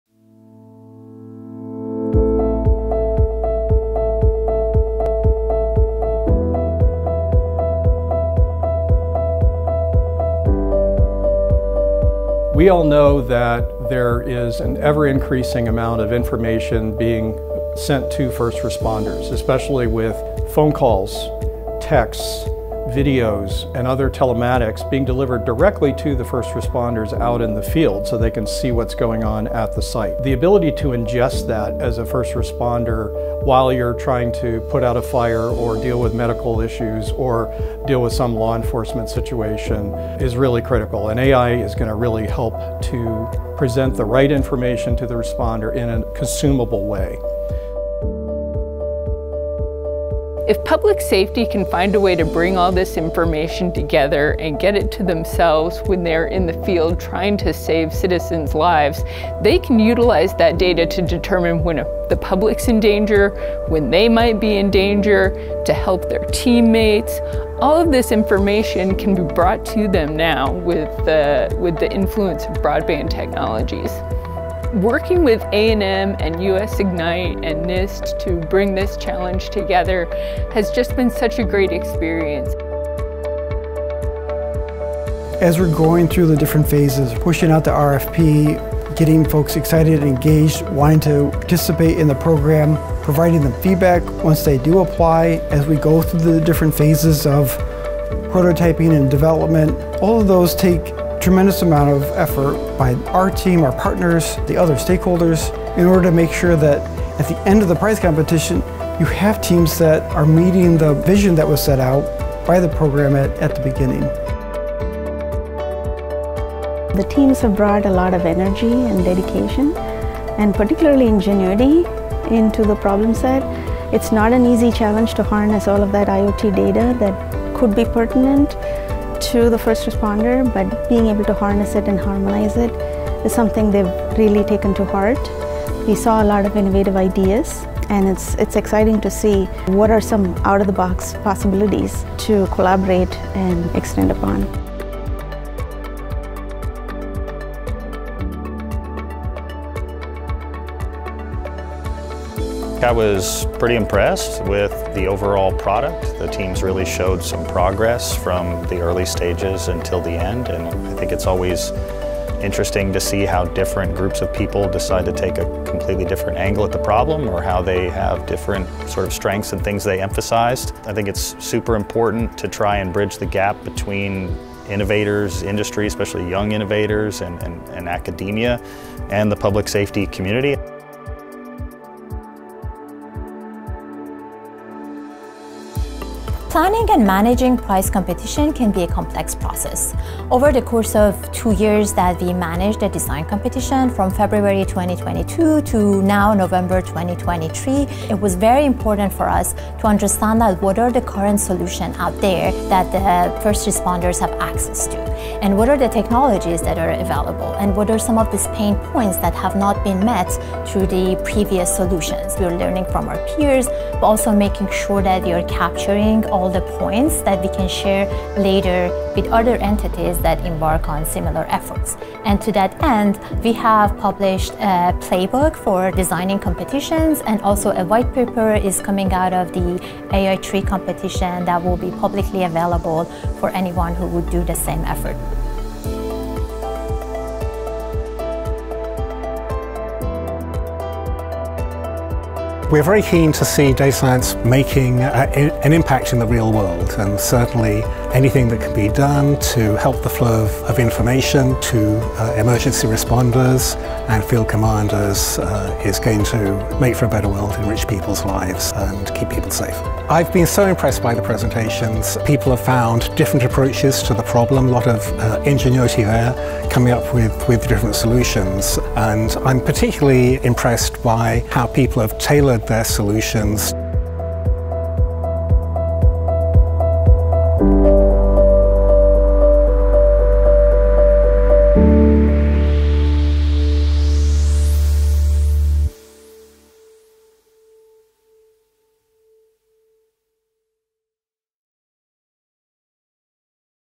AI3 Prize Competition: Organizing for Innovation in Public Safety
Watch to dive into the journey of organizing the AI for IoT Information (AI3) Prize Competition, a collaborative effort led by US Ignite, Texas A&M University, and TEEX with financial support from NIST. The AI3 Prize Competition brought together researchers, developers, students, and AI enthusiasts to create AI-driven solutions that empower first responders.

In this video, some of the AI3 Prize Competition’s key organizers discuss the meticulous planning involved in the prize competition, from drafting the problem statement to guiding the teams through the various phases of development. US Ignite’s  “A Communities Guide to Challenges and Competitions” is highlighted as a foundational guiding resource for replicating similar efforts.

The discussions emphasize how the AI3 Prize Competition fostered the integration of new technologies, like AI and 5G, into first responder workflows, enabling them to harness data effectively. Harmonizing and analyzing this information is crucial, allowing responders to make informed decisions in real-time.

Watch for highlights of the dynamic range of solutions created, demonstrating the participating teams' ingenuity and dedication. The AI3 Prize Competition's success can be a model for future initiatives exploring innovative, data-driven solutions that benefit first responders and public safety.